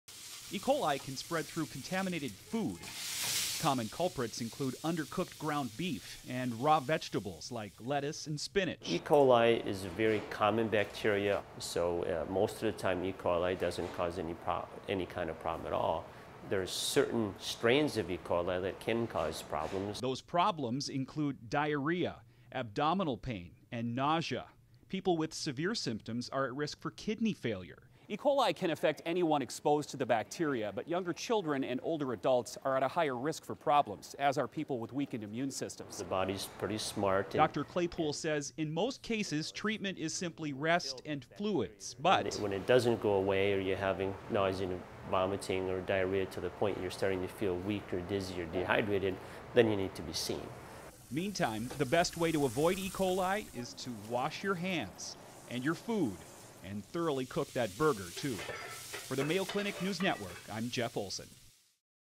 Mayo Clinic Minute: E. coli Fast Facts
The return of the summer barbecue brings with it the risk for sickness from E. coli in food that hasn’t been handled properly.

Most varieties of E. coli are harmless or cause brief sickness. However, a few particularly nasty strains, such as E. coli O157:H7, can cause more severe symptoms. And, unlike many other disease-causing bacteria, E. coli can create an infection even if you ingest only small amounts. Because of this, you can be sickened by E. coli from eating a slightly under cooked hamburger or a contaminated side salad.

In this Mayo Clinic Minute, reporter Jeff Olsen speaks with an emergency medicine doctor about common causes E. coli sickness and what can be done to prevent them.